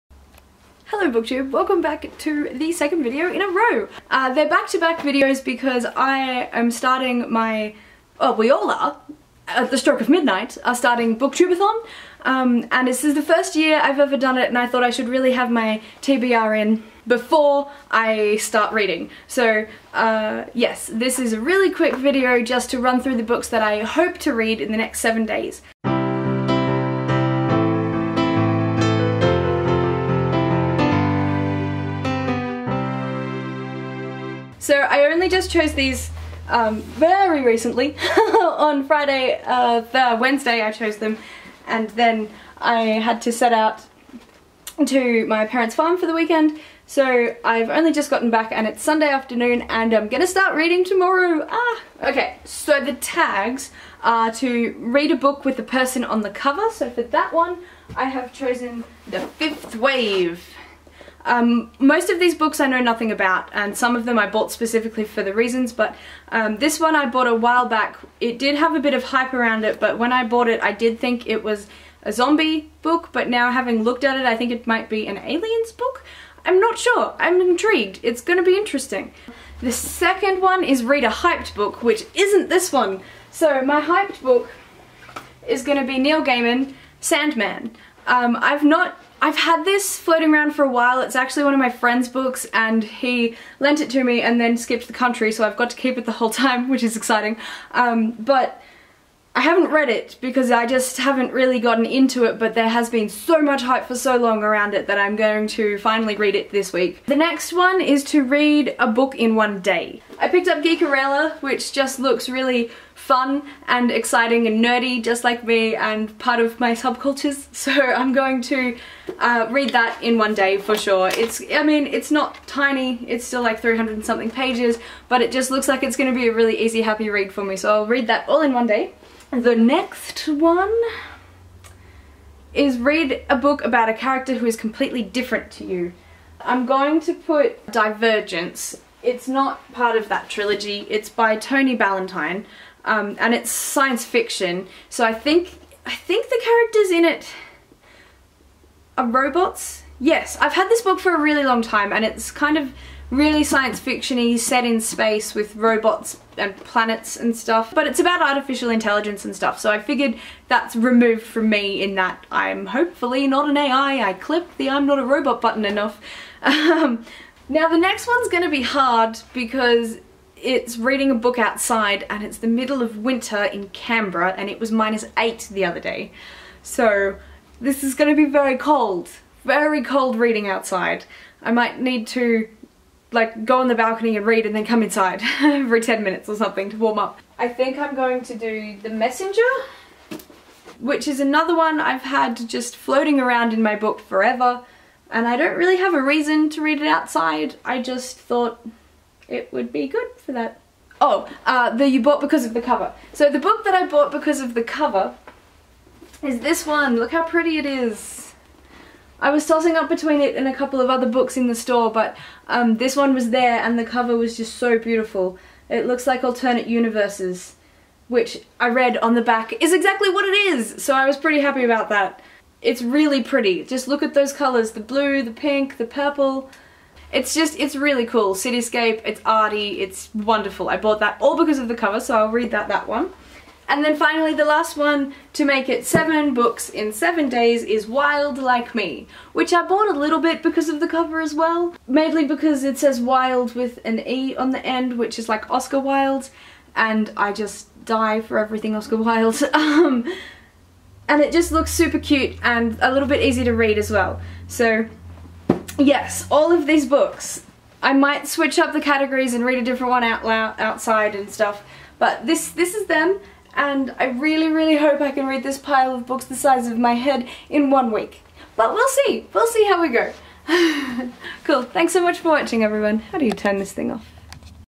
My First BookTube-A-Thon TBR
Books mentioned:   (in no particular order)
The 5th Wave by Rick Yancey
Geekerella by Ashley Poston
Divergence by Tony Ballantyne
The Messenger by Markus Zusak
A Thousand Peices Of You by Claudia Gray
Wilde Like Me by Louise Pentland
The Sandman by Neil Gaiman

-Span